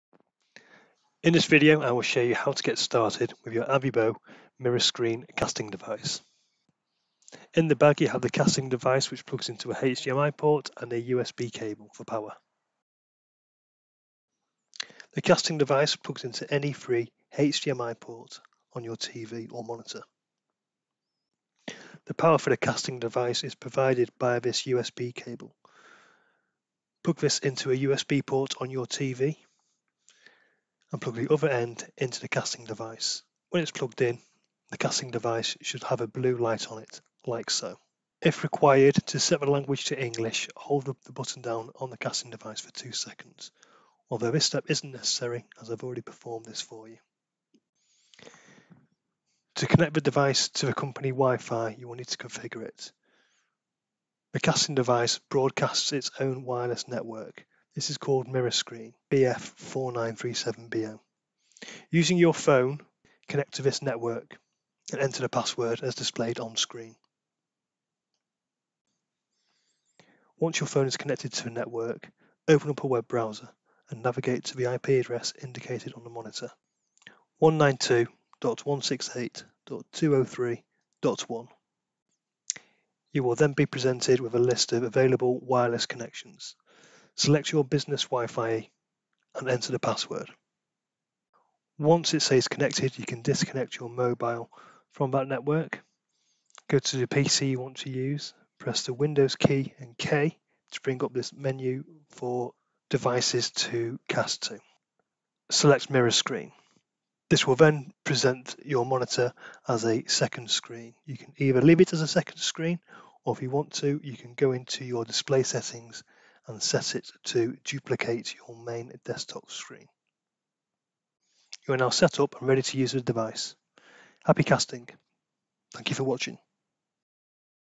AIVIBO Mirascreen Cast devise set up
How to set up the AIVIBO Mirascreen Cast devise.